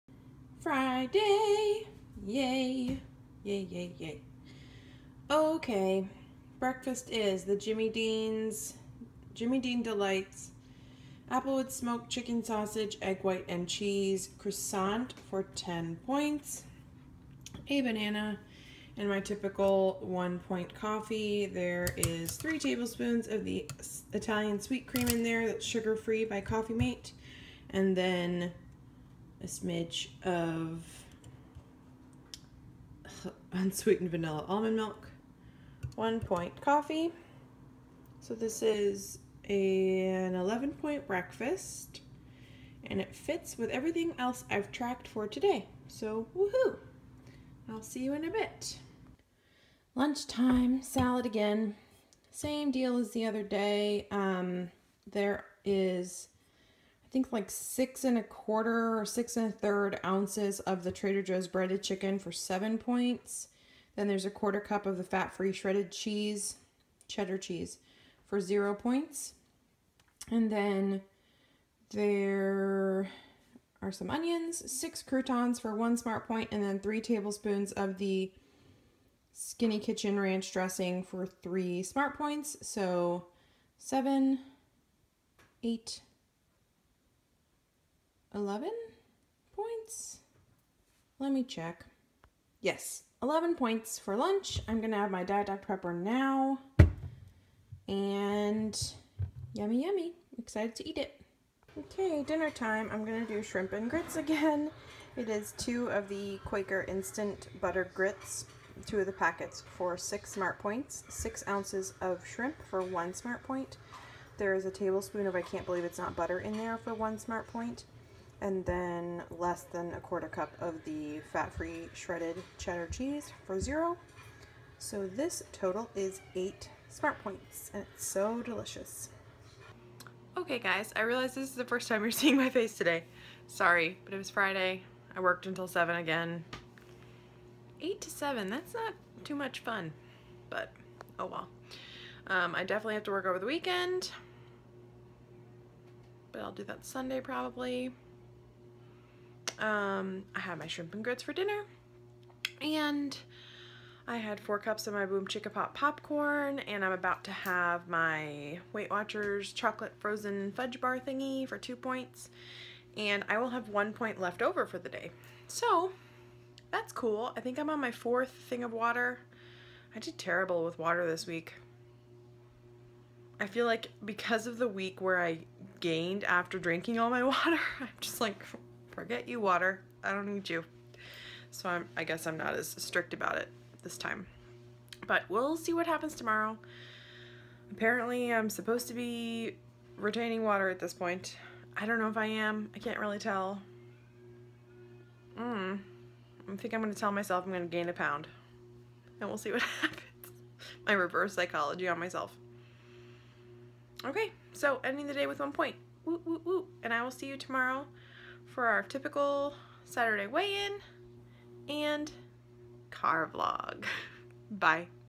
Weight Watchers | The Daily Eats - Smart Points 4/22/16
Daily Smart Points:  37
Breakfast:  11SP
Lunch:  11SP
Dinner:  8SP
Snacks:  6SP

36/37 Used
Weeklies Left:  11SP